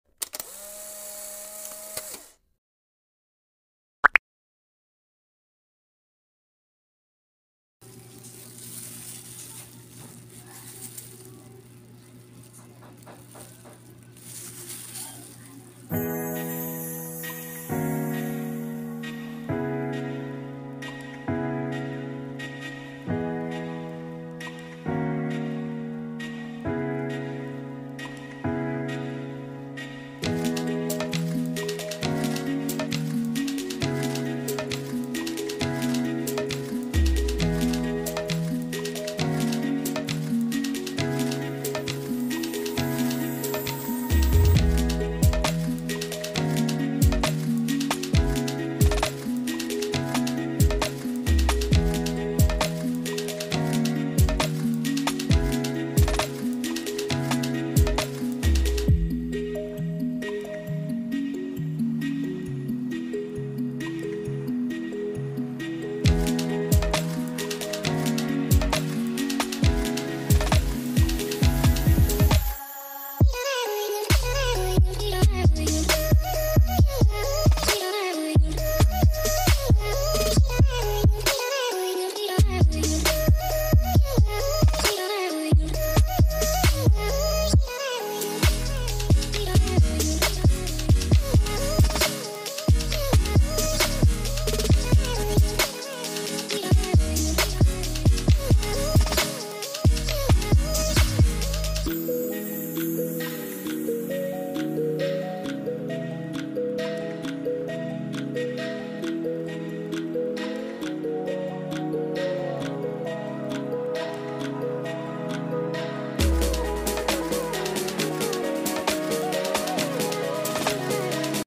EASY RECIPE KUTSINTA PANG NEGOSYO 5 ingredients only/ GmandGillian
kutsinta easy recipe
 ingredients

2 1/2 cups of  water
1 1/4 brown sugar
food coloring or assuete
3/4 Cup all purpose flour
11/4 cassava flour
1 tablespoons lye water